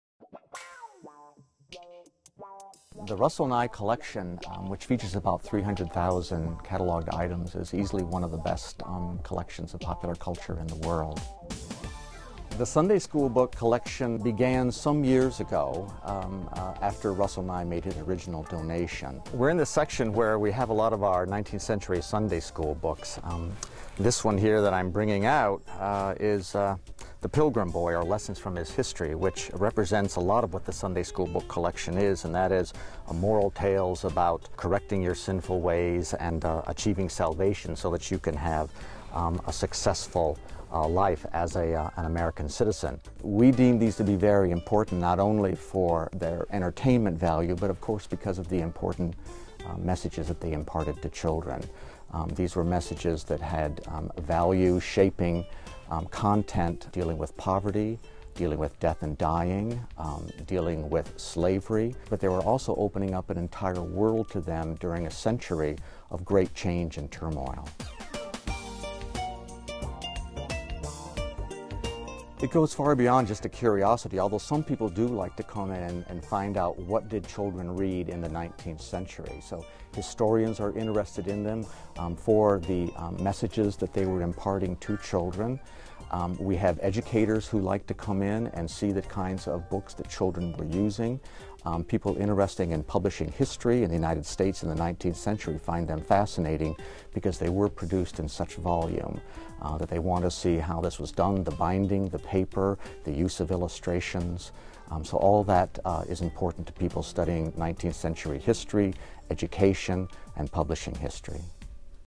Sunday School Books | MSU Libraries | PBS WKAR
The Sunday School Books Special Collection began shortly after Russel B. Nye's initial donation to MSU Special Collections. This collection contains books which can be described as "moral tales about correcting your sinful ways and achieving salvation so that you can have a successful life as an Amerian citizen."

With comments by Peter Berg, Head of Special Collections for the MSU Libraries.